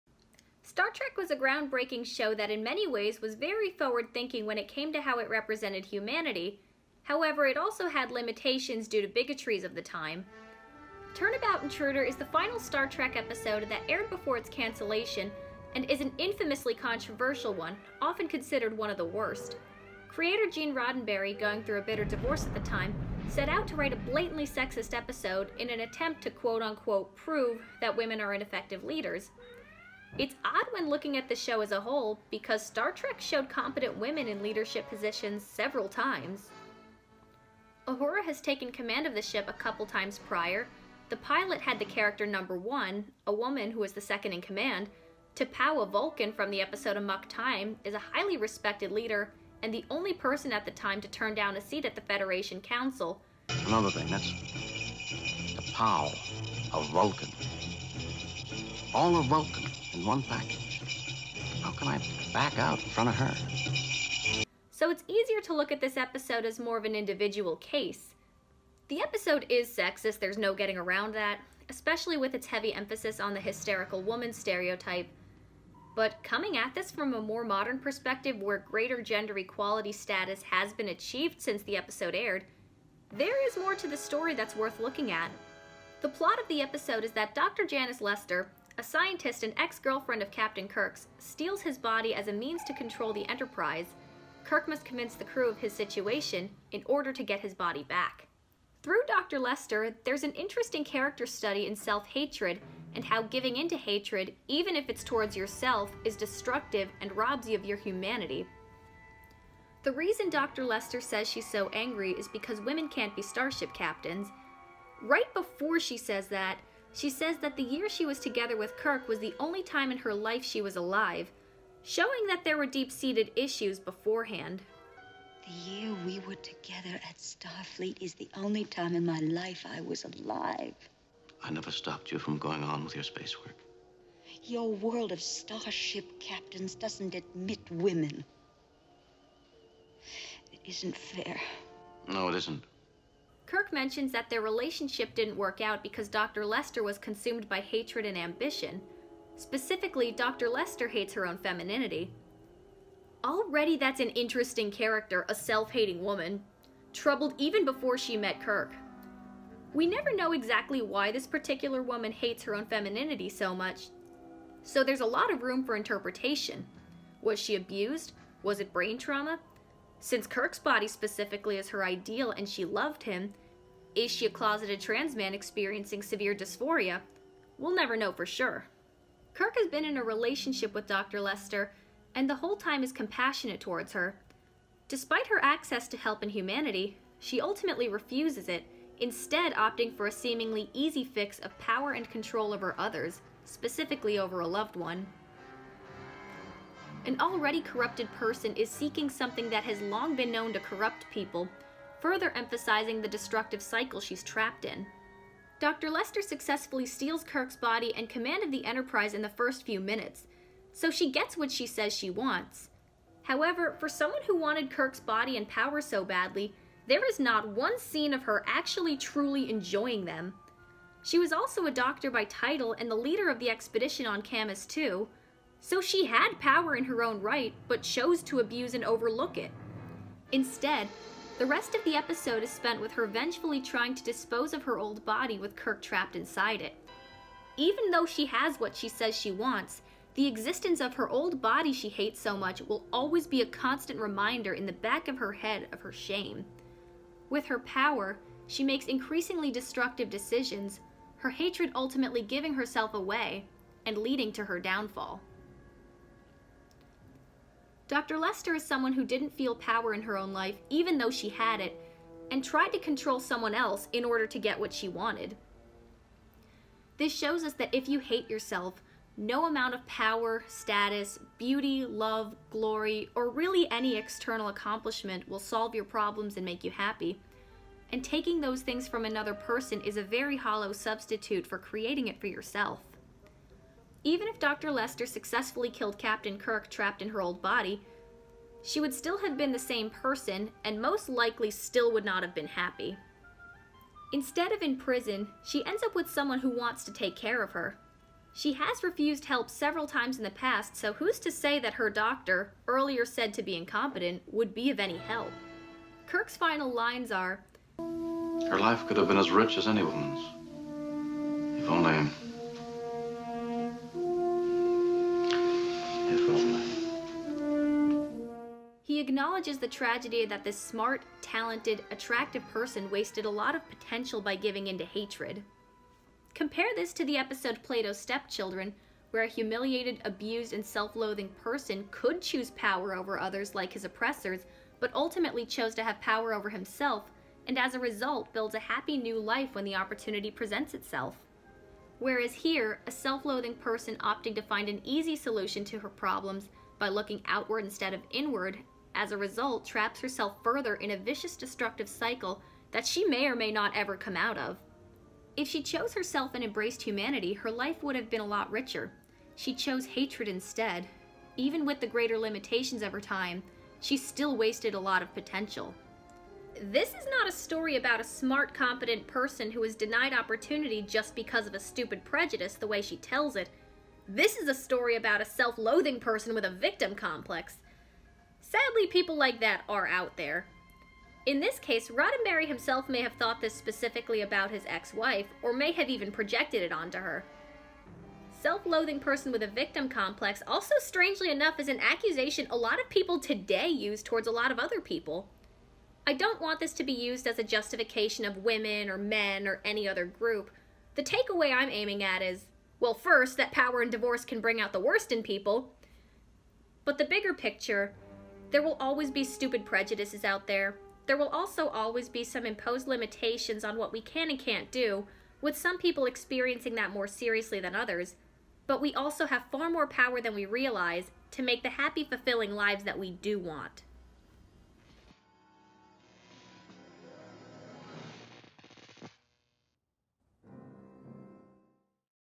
Turnabout Intruder Analysis: The Tragedy of Self Hatred
A controversial Star Trek episode with bitter intentions gives us a fascinating character study in self-hatred. In it, Captain Kirk's ex steals his body in order to command the Enterprise.